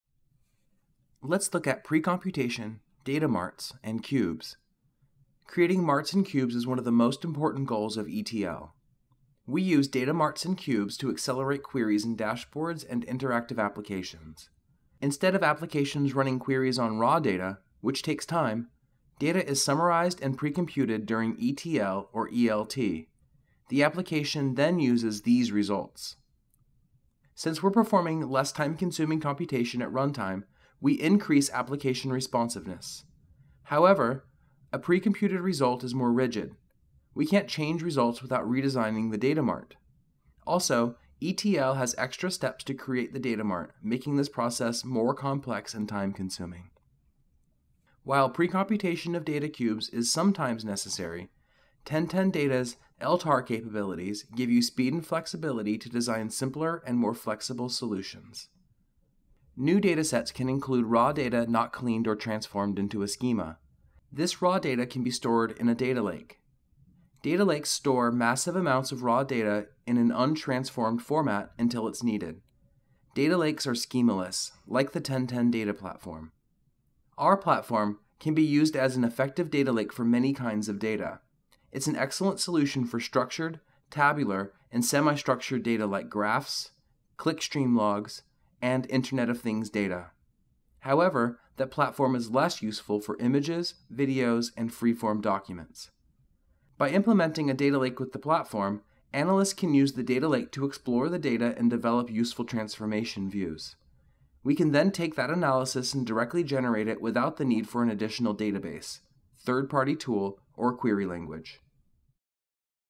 1.5 - Data Marts, Cubes, and Lakes
This video covers the following:

Data marts and cubes
o Accelerate queries

Data lakes
o Store raw data until needed
o Structured, tabular, semi-structured data
o Graphs, clickstream logs, internet of things